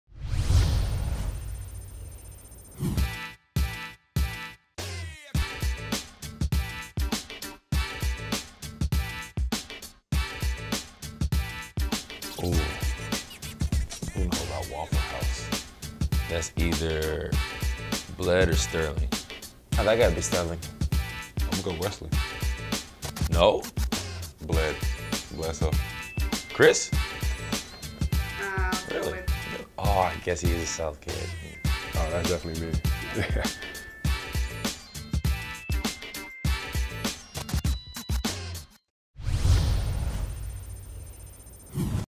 Who tweeted it: Waffle House
Which Milwaukee Bucks player tweeted about Waffle House? Giannis Antetokounmpo and his teammates try to guess.